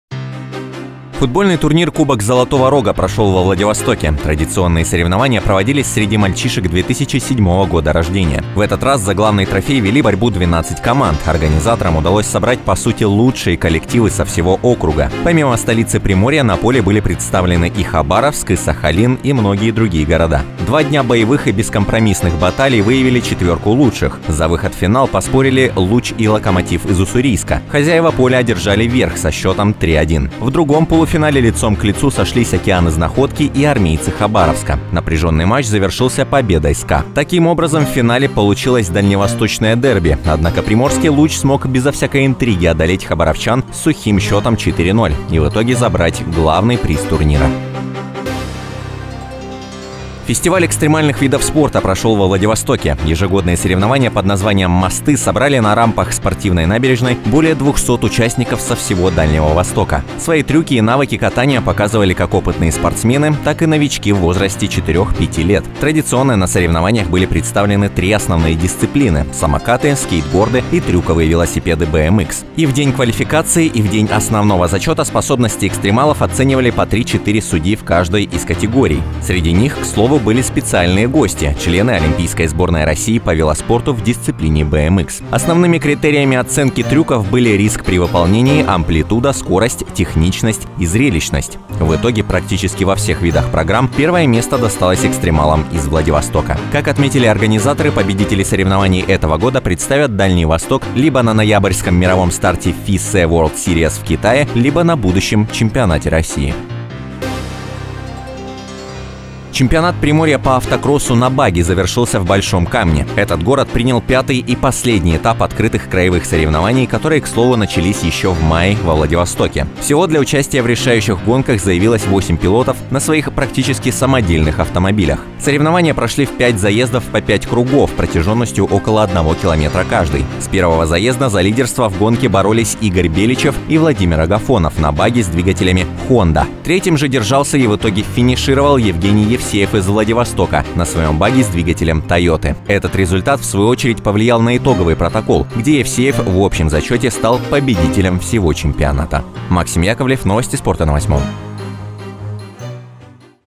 ЛУЧШИЕ В ТРЮКАХ: ОБЗОР СПОРТА
Юные футболисты со всего Дальнего Востока поборолись за "Кубок Золотого Рога".  Экстрим-фестиваль "Мосты" определил лучших трюкачей. Любители гонок на багги подвели итоги чемпионата Приморья. Подробности расскажет Максим Яковлев.